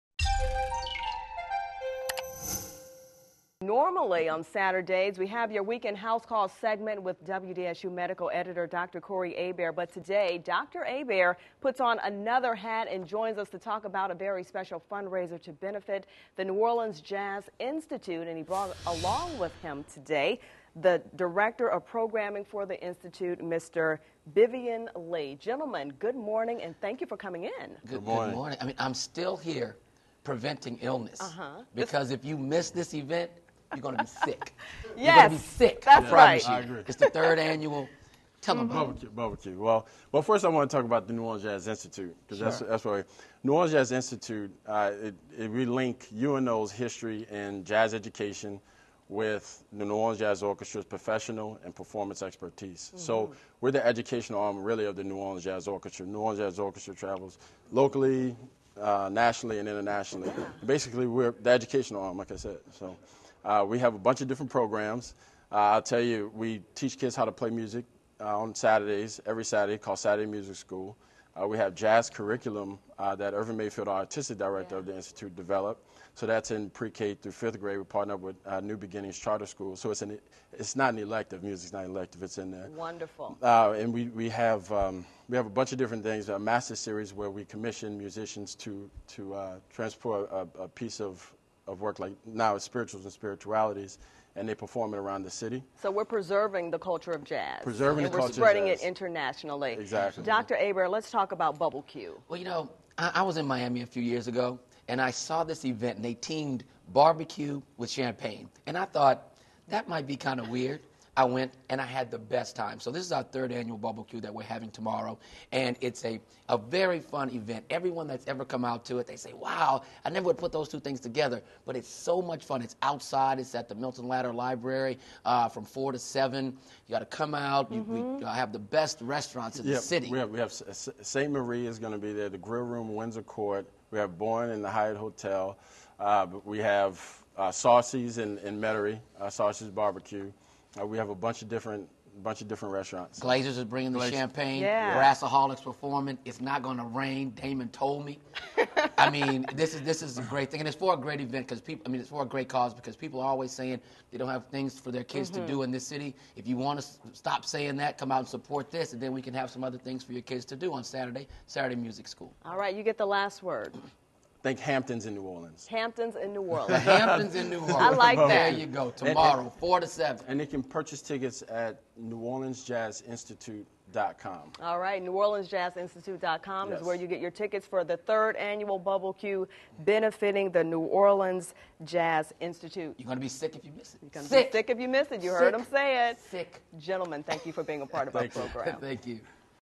Event Combines BBQ And Bubbly
BubbleQ combines bubbly and BBQ to benefit the New Orleans Jazz Institute.